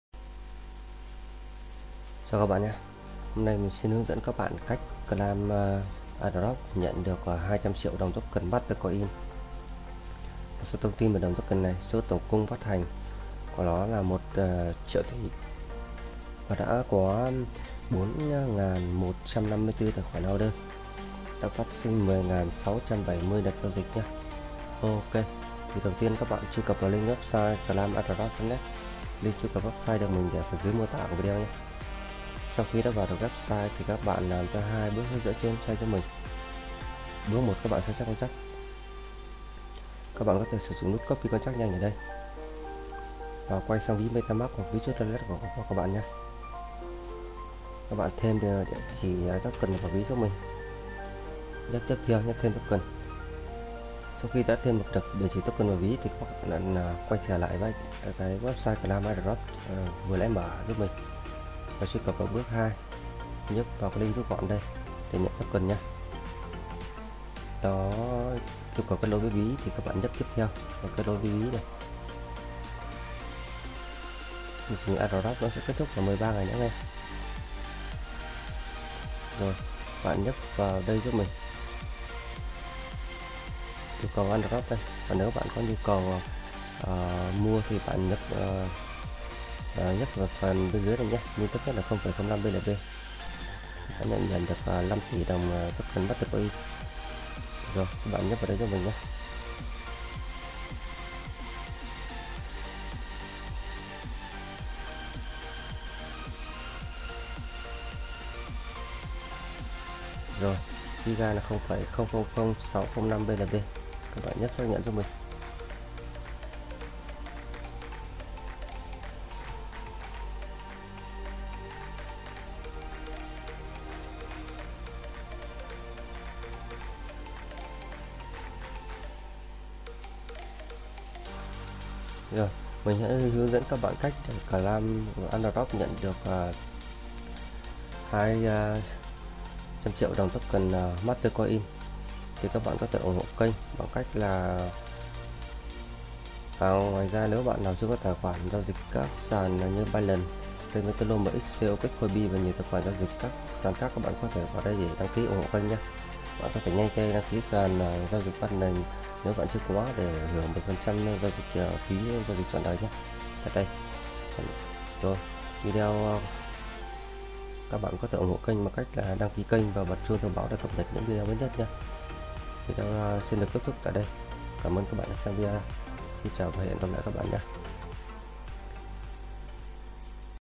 Claim Airdrop Nhận 200.000.000 Token Master Coin (MRC), Mạng BSC Ví Trustwallet, MetaMask
Claim Airdrop Nhận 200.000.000 Token Master Coin (MRC), Mạng BSC Ví Trustwallet, MetaMask Phí 0,0003 BNB

+ Bước 1: Sao chép Contract :  0xE77C19d2113b3a1b1b49b71eB39954820ab23cFE

+ Bước 2: Truy Cập Web Tại Đây Click ( Claim Airdrop ) Nhận Token